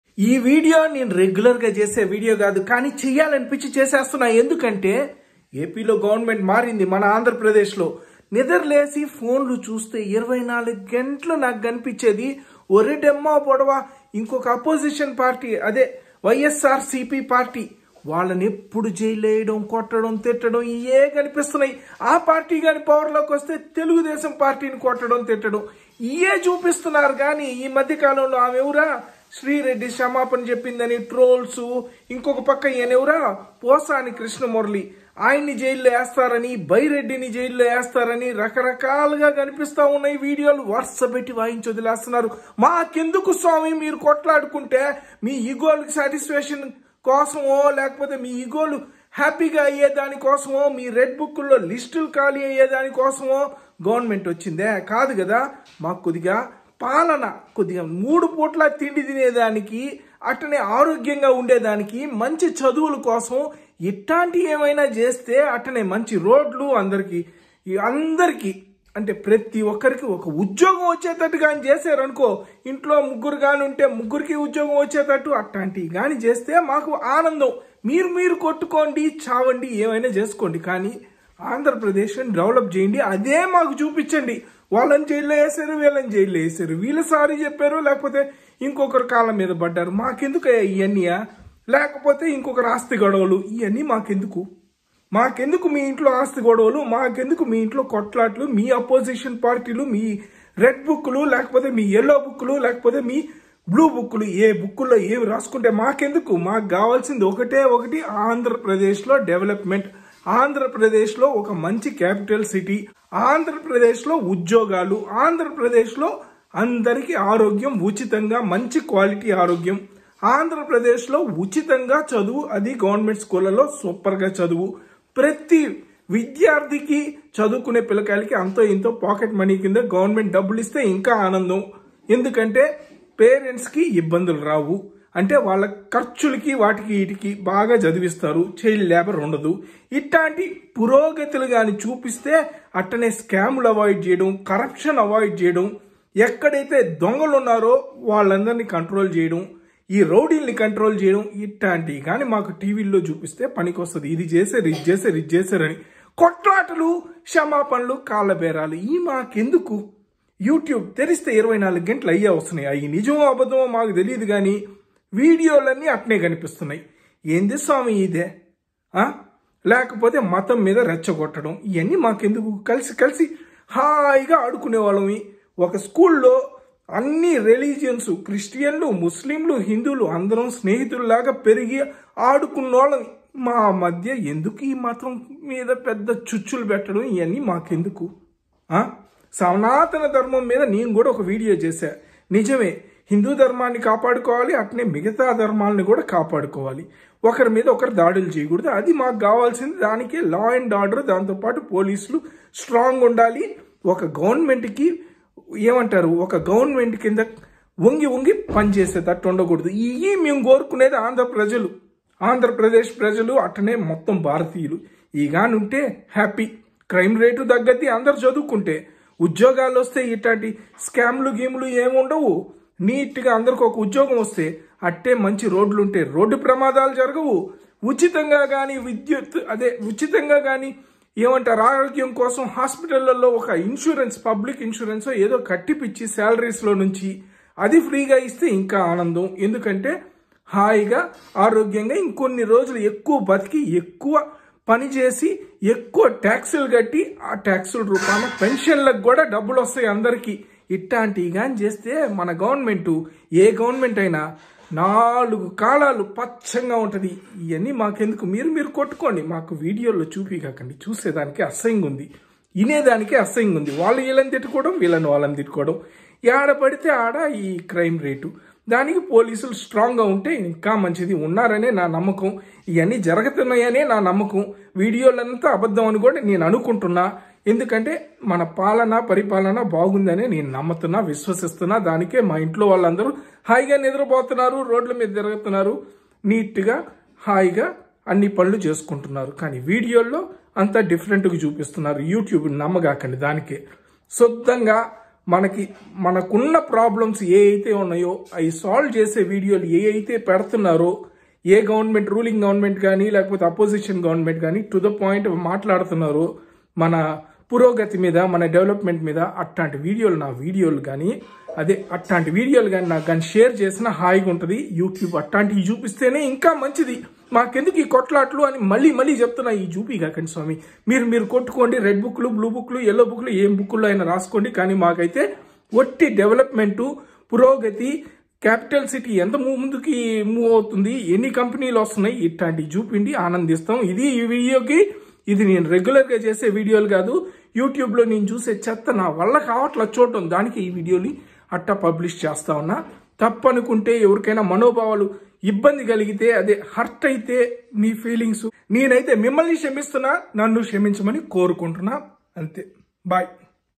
Andhra political revenge videos ki oka dannam | AP Politics
YouTube stop showing us such political trolls about Andhra Politics and do not disturb our minds by watching scrap and scripted videos to defame our state .

Show only reality of development that our respected leaders are working on 🙏